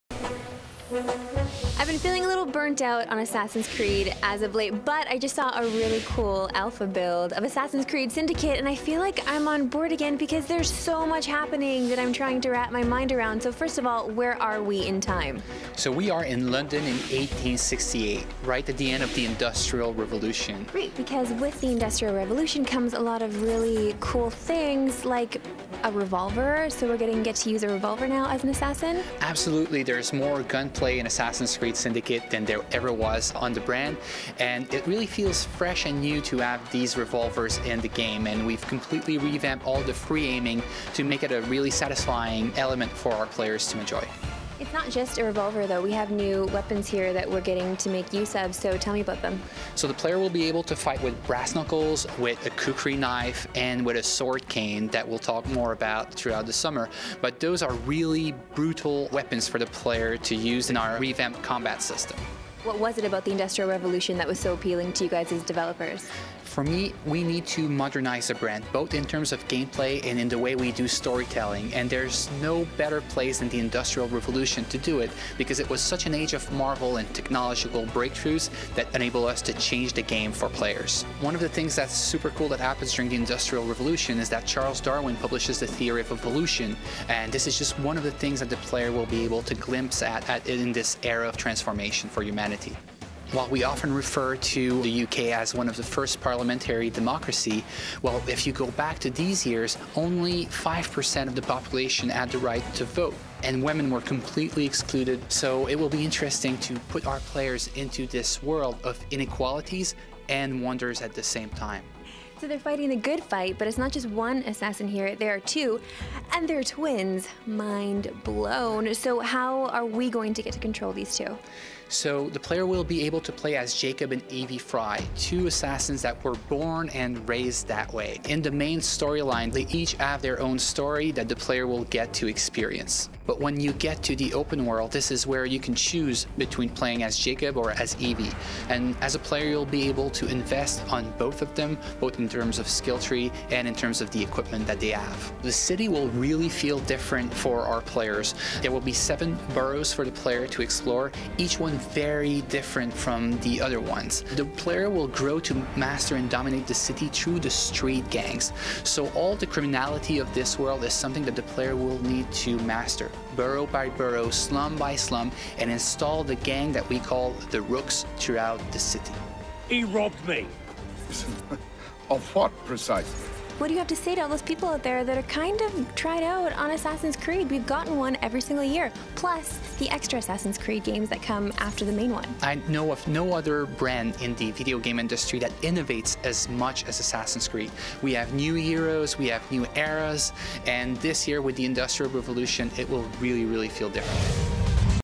Assassin's Creed Syndicate - Developer Interview
Assassin's Creed Syndicate was just announced this morning, but Marissa already has an early look with creative director Marc-Alexis Côté at Ubisoft Quebec.